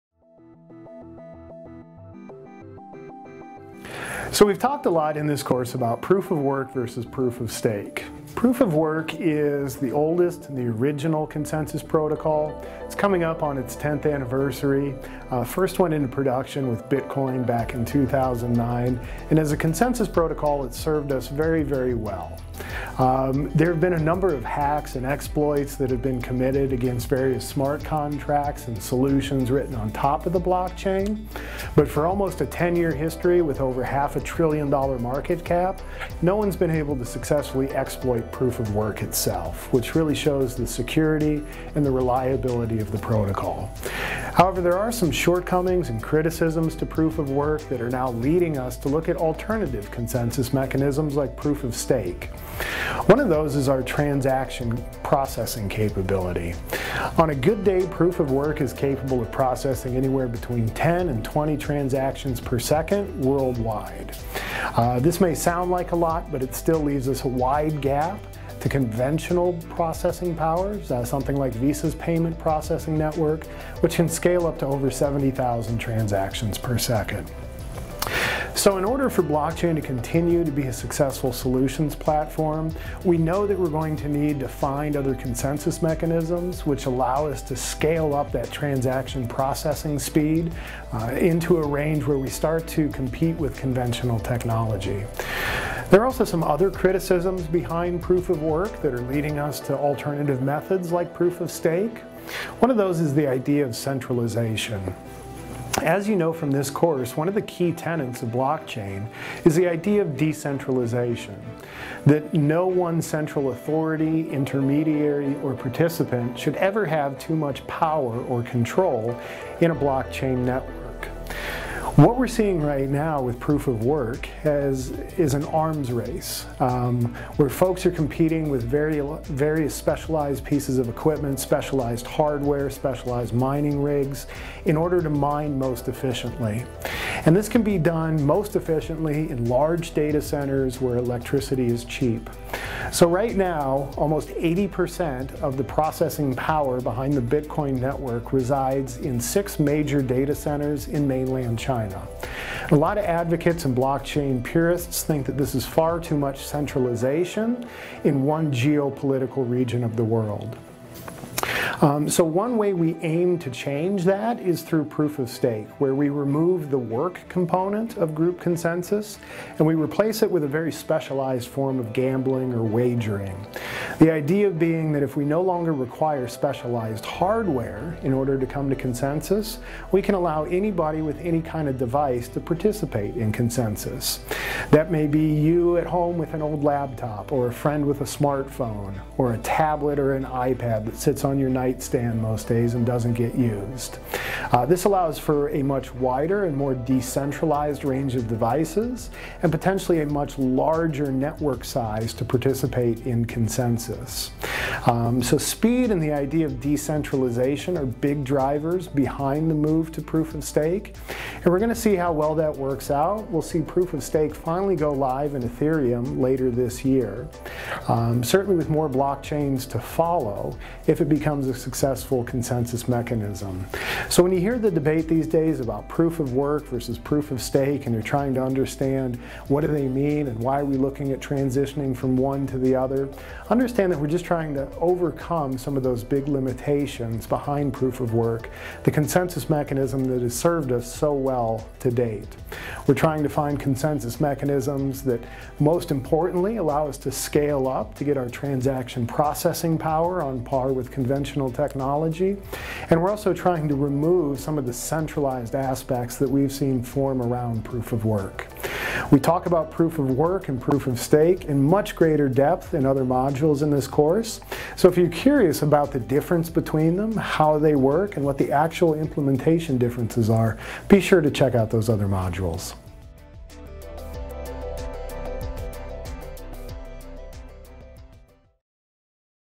Proof of Work vs. Proof of Stake
This video is about Proof of Work vs. Proof of Stake.
So, we've talked a lot in this course about Proof of Work versus Proof of Stake. Proof of Work is the oldest and the original consensus protocol. It's coming up on its tenth anniversary; first one into production with Bitcoin back in 2009.